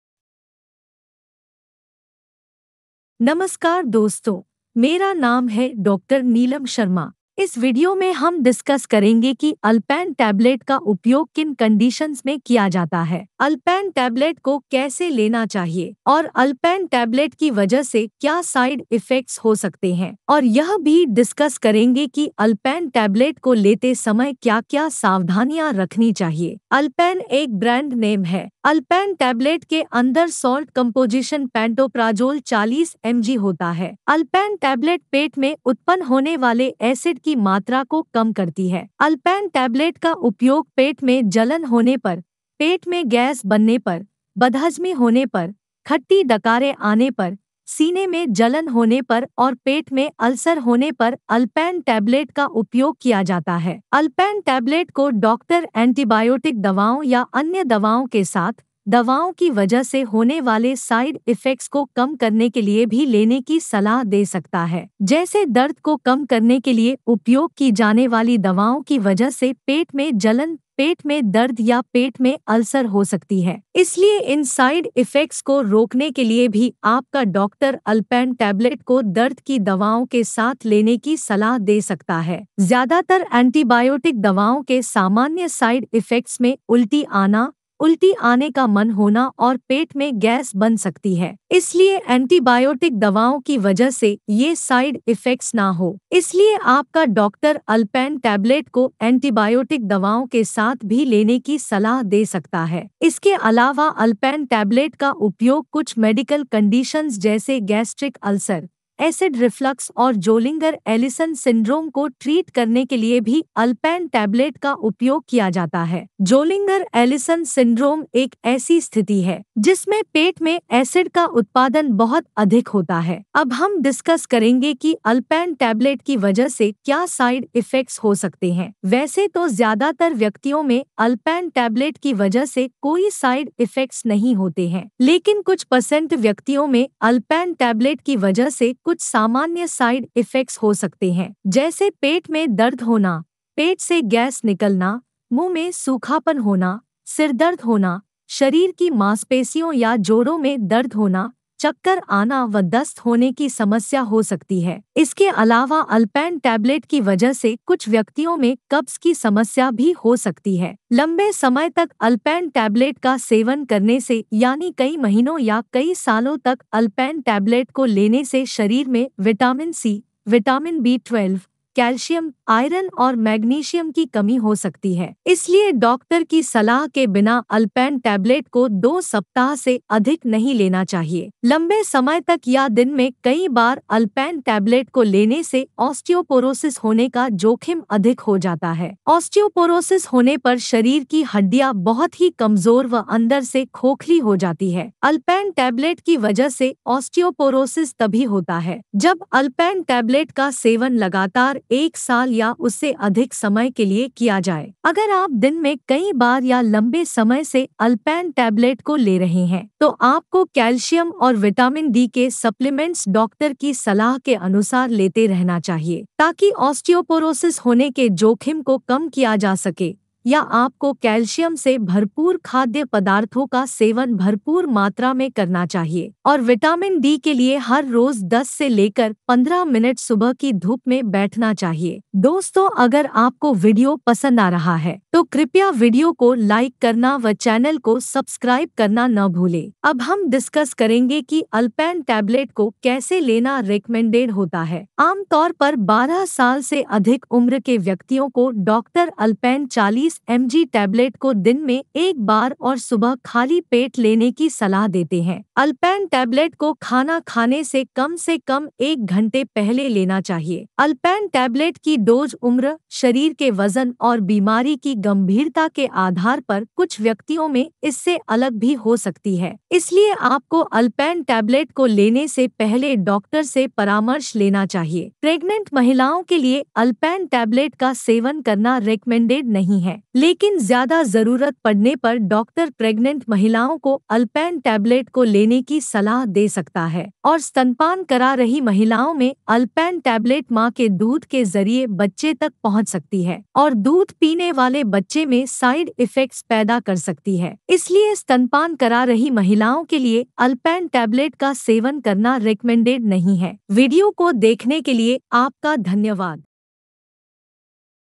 Ulpan Tablet
in this video, we will discuss what are the uses and benefits of ulpan tablet. How to use ulpan tablet. We will also discuss what are the side effects of ulpan tablet. Watch the complete video for detailed review of ulpan tablet in hindi.